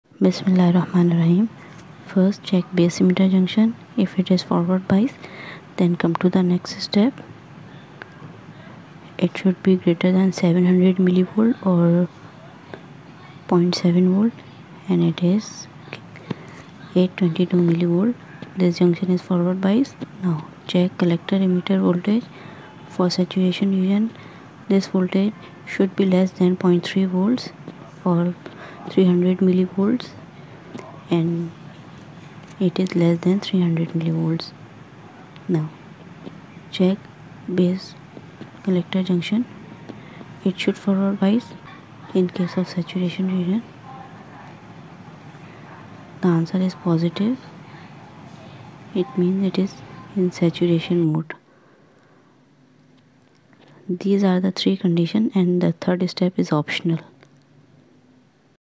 BJT Saturation Mode Conditions Using Multisim Live
How to test BJT Saturation Mode using Multisim Live?
Testing a BJT is simple. There are two steps:

1. Calculate base to emitter voltage (VBE,) for an NPN transistor. it should be greater than 0.7V.
2. Check the collector to emitter the junction voltage (VCE), it should be less than 0.3V.

Read written tutorial with step by step solved examples on different modes of BJT. How to identify different BJT modes?

# npn transistor
bipolar junctiontransistor